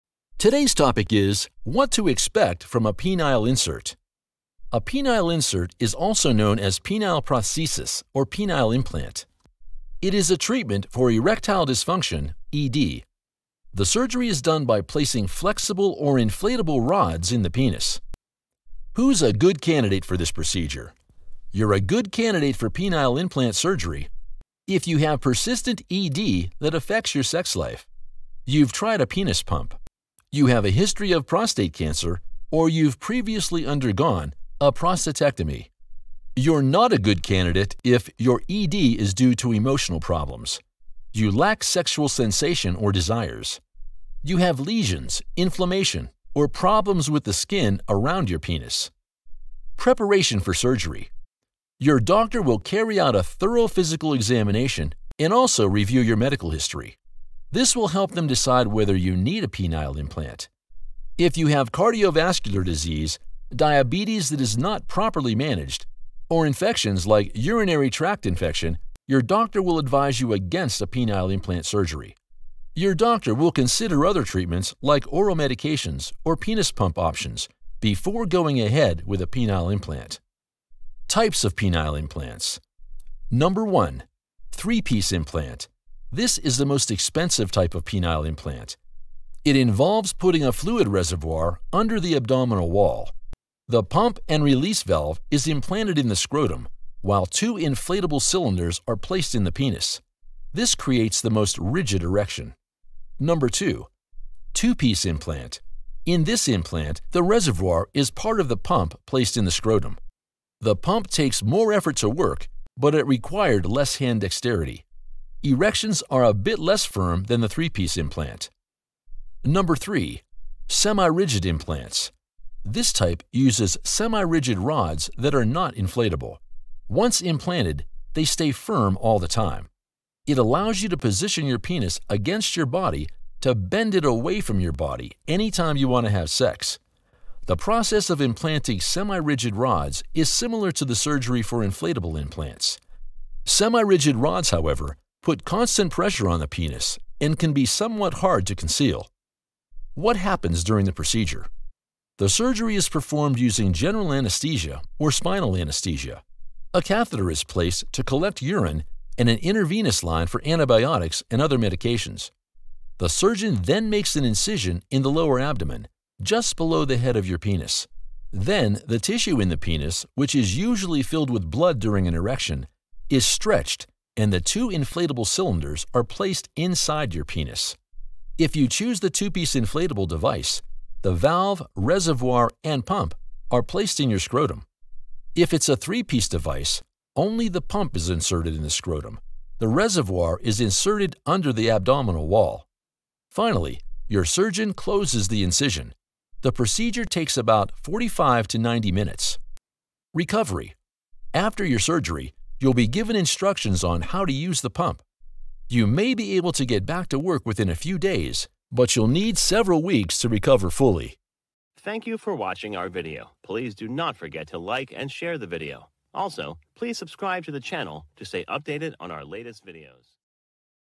What To Expect From A Penile Insert
Chapters

0:00 Introduction
0:20 Who is a good candidate for this procedure.
0:37 You are not a good candidate If
0:51 Preparation for surgery
1:21 Types of penile implants
1:24 Three- Piece Implant
1:44 Two-piece implant
2:01 Semirigid implants
2:31 What happens during the procedure
3:20 Recovery

A penile implant is an implanted device intended for the treatment of erectile dysfunction, Peyronie's disease, ischemic priapism, deformity and any traumatic injury of the penis, and for phalloplasty or metoidioplasty, including in gender-affirming surgery. Men also opt for penile implants for aesthetic purposes. Men’s satisfaction and sexual function is influenced by discomfort over genital size which leads to seek surgical and non-surgical solutions for penis alteration.[1] Although there are many distinct types of implants, most fall into one of two categories: malleable and inflatable transplants.[2]The first modern prosthetic reconstruction of a penis is attributed to NA Borgus, a German physician who performed the first surgical attempts in 1936 on soldiers with traumatic amputations of the penis. He used rib cartilages as prosthetic material and reconstructed the genitals for both micturition and intercourse purposes.[3] Willard E. Goodwin and William Wallace Scott were the first to describe the placement of synthetic penile implants using acrylic prosthesis in 1952.[4] Silicone-based penile implants were developed by Harvey Lash and the first case series were published in 1964.[5] The development of a high-grade silicone that is currently used in penile implants is credited to NASA.[6] The prototypes of the contemporary inflatable and malleable penile implants were presented in 1973 during the annual meeting of the American Urological Association by two groups of physicians from Baylor University (Gerald Timm, William E. Bradley and F. Brantley Scott) and University of Miami (Michael P. Small and Hernan M. Carrion).[3][7][8] Small and Carrion pioneered the popularization of semi-rigid penile implants with the introduction of Small-Carrion prosthesis (Mentor, USA) in 1975. Brantley Scott described the initial device as composed of two inflatable cylindrical bodies made up of silicone, a reservoir containing radiopaque fluid and two pumping units.[7] The first generation products were marketed through American Medical Systems (AMS; currently Boston Scientific), with which Brantley Scott was associated.[6][9] Many device updates have been released by AMS since the first generation implants. In 1983, Mentor (currently Coloplast) joined the market.[6] In 2017, there were more than ten manufacturers of penile implants in the world, however only a few now remain in the market.[10] The latest additions to the market are Zephyr Surgical Implants and Rigicon Innovative Urological Solutions.[11][12] Zephyr Surgical Implants, along with penile implants for biological men, introduced the first line of inflatable and malleable penile implants designed for sex reassignment for trans men.

According to analysis of the 5% Medicare Public Use Files from 2001 to 2010 approximately 3% of patients diagnosed with erectile dysfunction opt for penile implantation.[13] Each year nearly 25,000 inflatable penile prostheses are implanted in the USA.[14]

The list shows penile implants available in the market in 2020. Malleable penile implant
Comparison of penile implants (inflated cylinder diameter and length)
Penile implant pump design comparison

The malleable (also known as non-inflatable or semi-rigid) penile prosthesis is a set of a pair of rods implanted into the corpora of the penis. The rods are hard, but 'malleable' in the sense that they can be adjusted manually into the erect position.[16] There are two types of malleable implants: one that is made of silicone and does not have a rod inside, also called soft implants, and another with a silver or steel spiral wire core inside coated with silicone. Some of the models have trimmable tails intended for length adjustment.[10] Currently, a variety of malleable penile implants are available worldwide.[17]
Inflatable penile implant